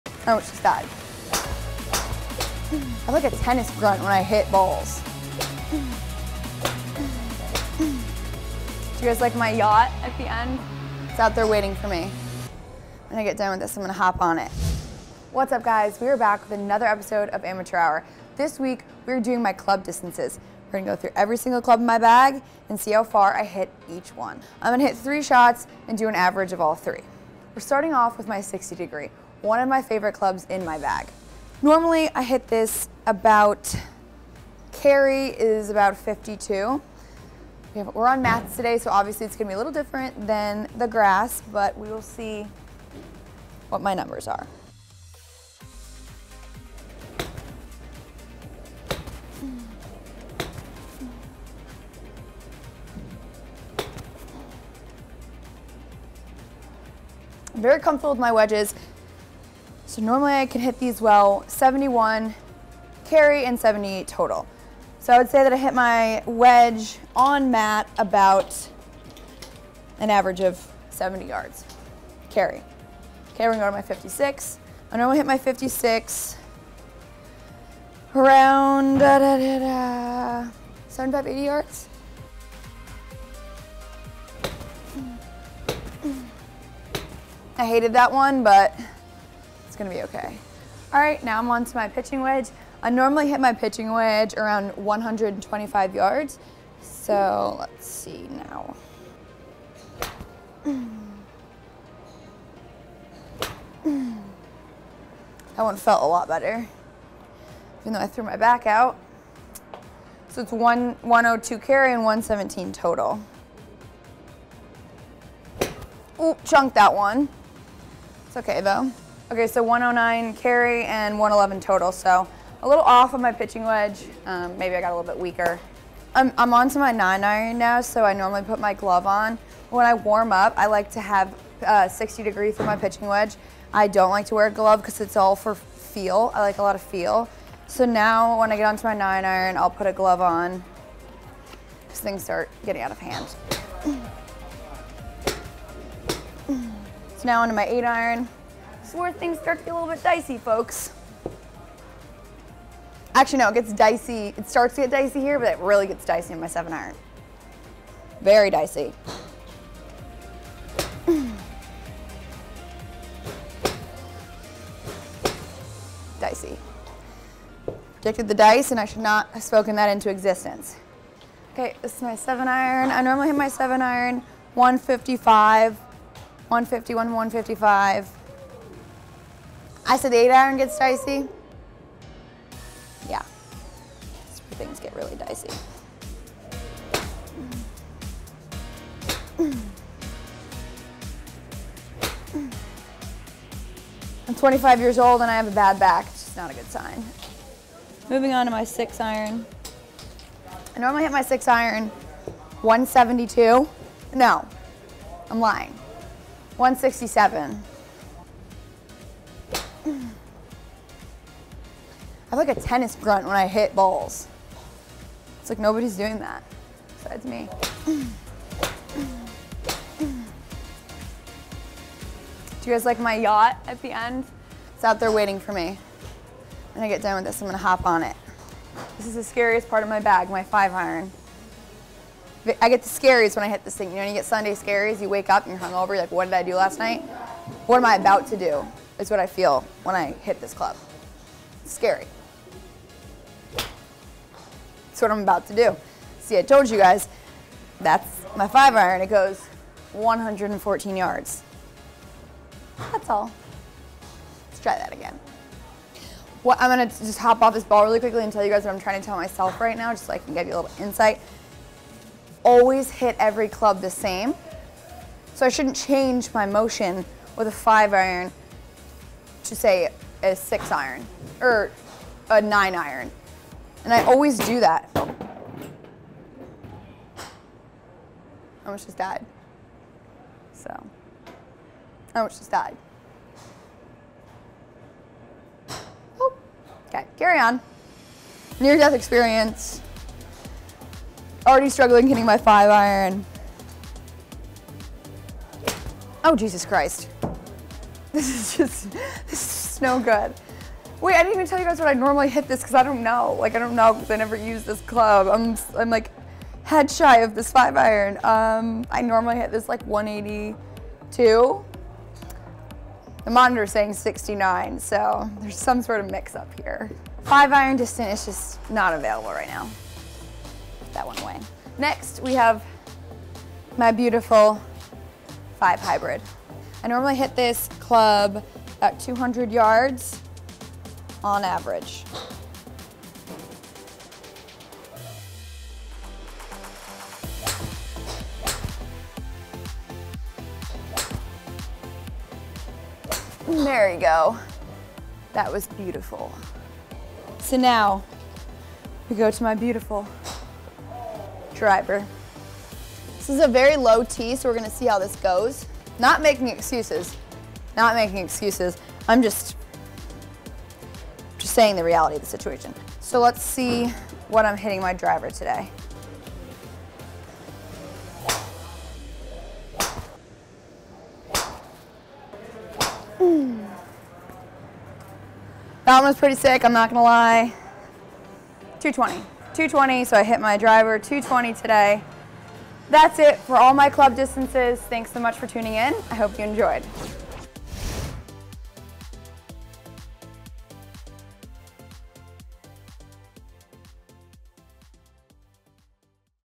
Figuring Out Your Club Distances | Amateur Hour Golf
Hannah figures out her club distances.